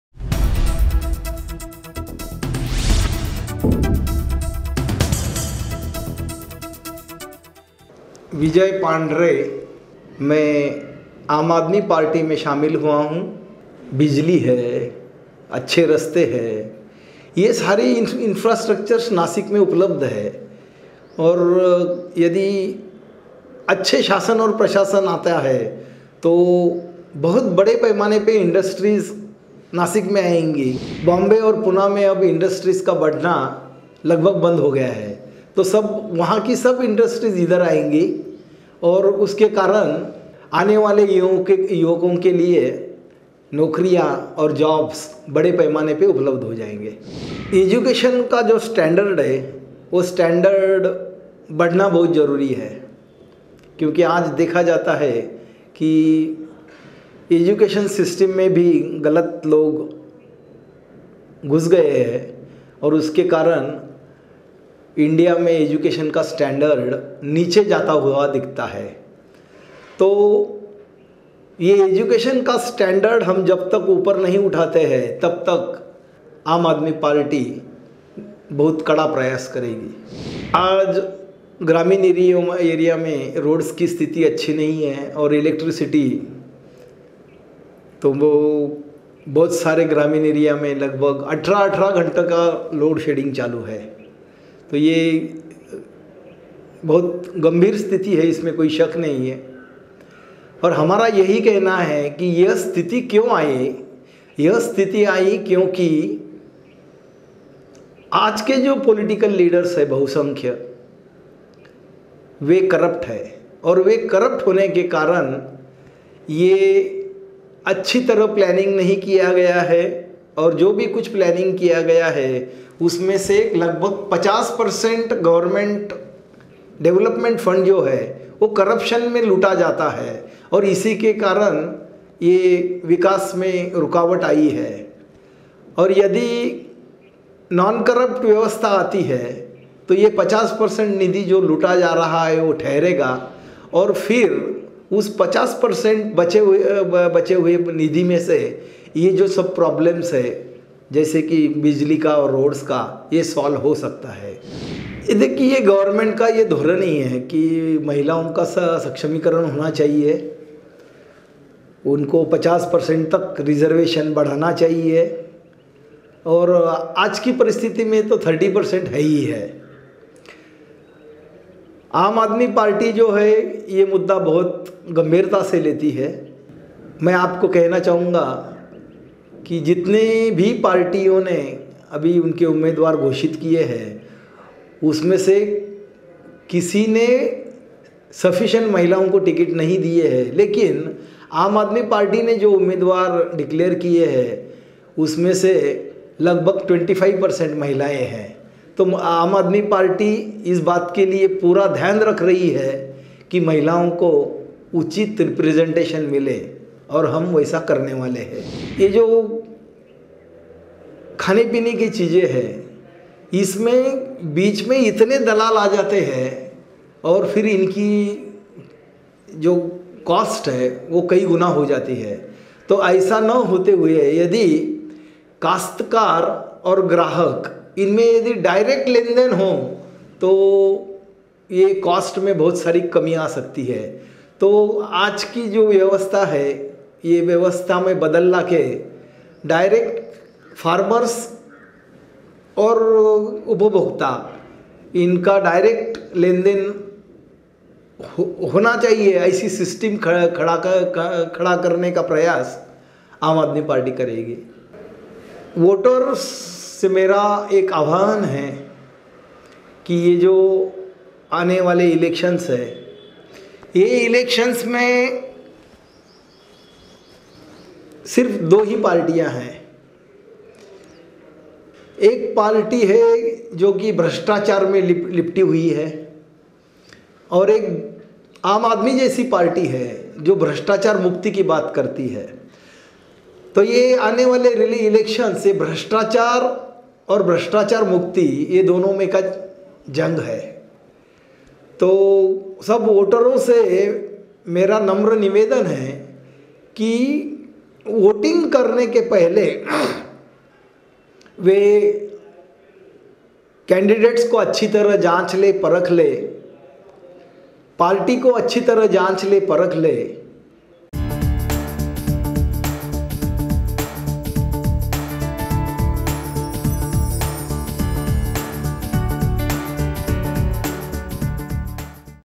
Vijay Pandhare, AAP || Nashik, Maharashtra
Vijay Pandhare
Constituency: Nashik, Maharashtra
Party: AAP
Status: Opposing Candidate
Age: 53